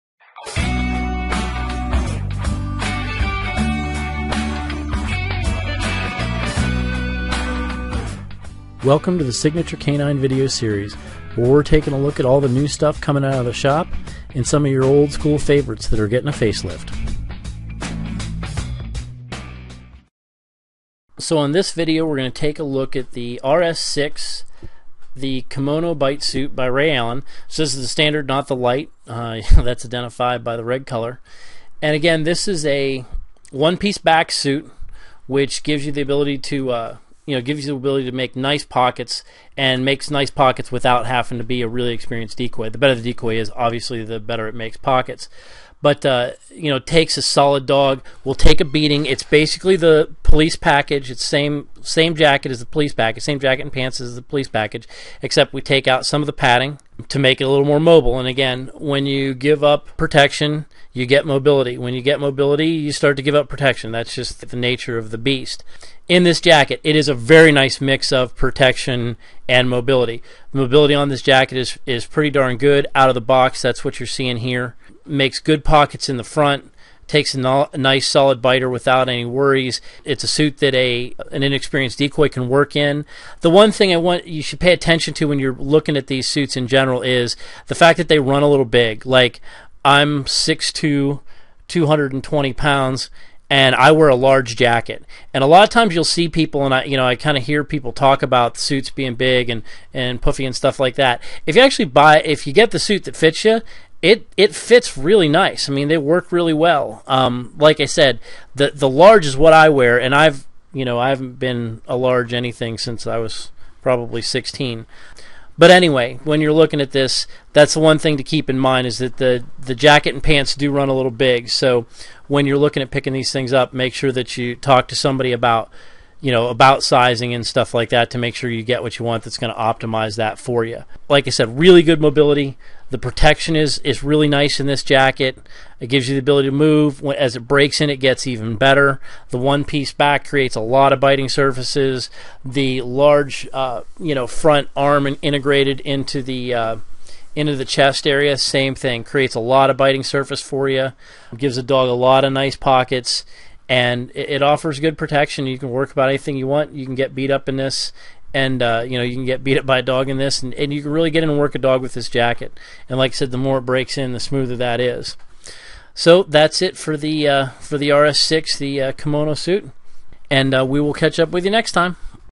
Ray Allen RS6 Bite Suite
Ultra Kimono Training Bite Suit

Reduce fatigue with this lightweight and flexible bite suit. Perfect for dog's trained in KNPV.

These bite suits are perfect for experienced decoys and working with Dutch KNPV trained dogs. Special attention is given to the armpit and upper arm areas to provide world class protection as well as a very generous bite surface for the K-9 in training. High-back and chest bites can be taken with confidence. All of our Ultra Kimono Bite Suits feature flexible arm and knee construction to allow for extreme range of motion. Unique design hides closures and gussets, preventing tooth damage. The quick release closures, snaps and wide leg design offers Easy On-Easy Off dressing. The Pant Zipper Option allows you to keep your shoes on while putting the bite suit pants on or taking off. And, conveniently, the whole bite suit is machine washable.

Approximate sizes are as follows; Medium fits 5'8" to 5'10" up to 175 lbs. Large fits 5'11" to 6'2" up to 220 lbs. X-Large fits 6'2" to 6'5" up to 250 lbs. For sizing instructions, click Bite Suit Sizing.

Due to the benefits of a good fit and and other key details, Ray Allen Bite Suits are only available through phone order at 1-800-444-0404. Please call with any questions and to order.